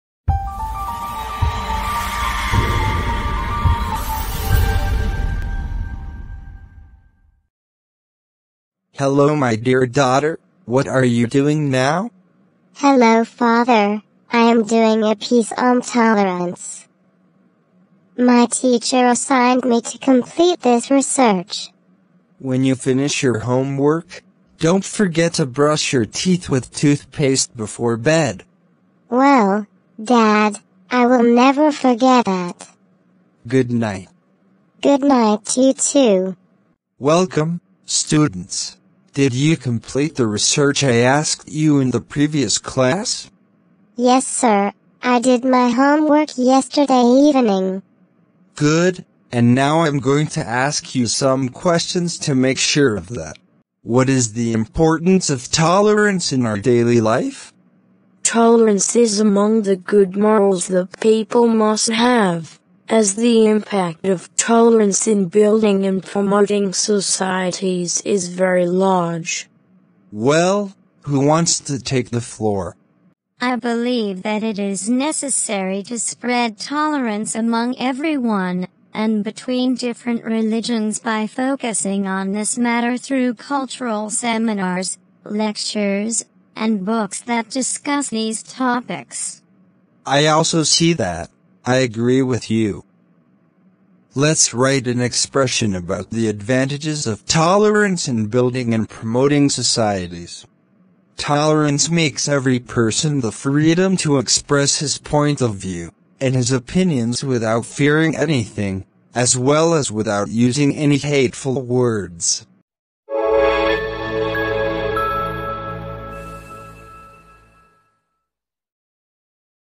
A short story about tolerance among people 👍
This is a short story about tolerance
 Tolerance has great effects on the individual and society, it is a good tree that bears fruit.